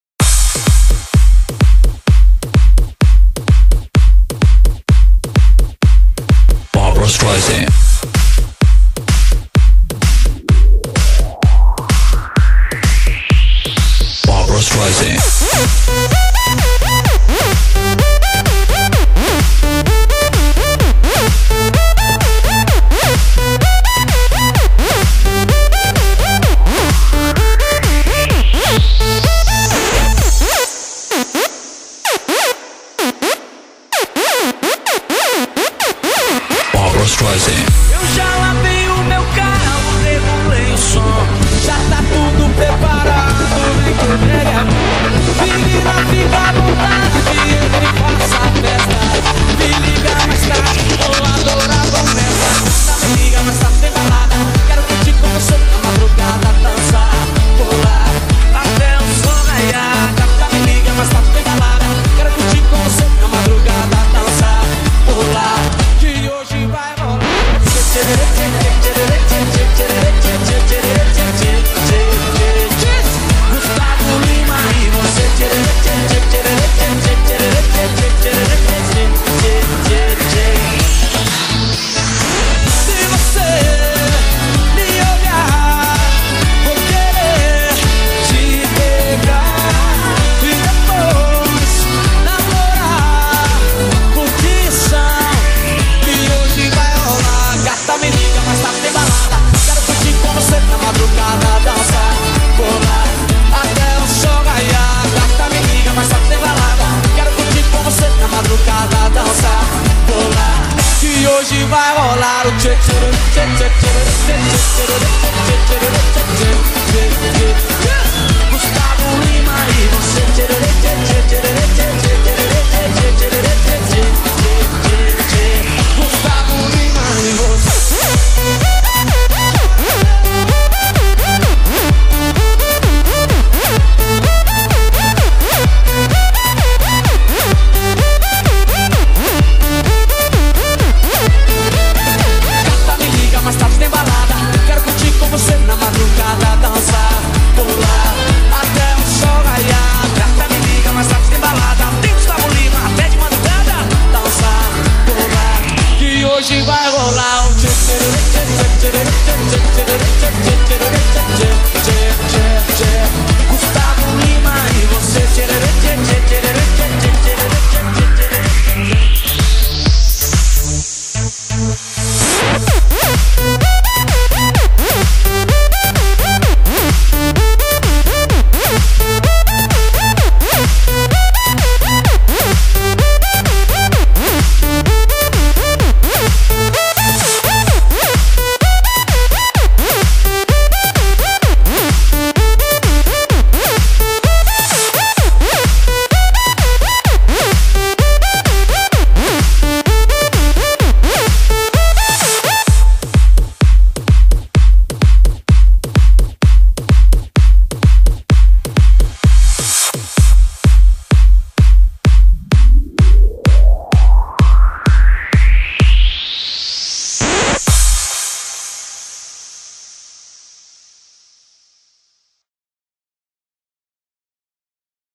dj alissonleo galoucura de lafaiete!!
essse video e para a galera q curti un sertanejo remixado!!
e en homenagem aos atleticanos!!
vamo pra cima deles galo!!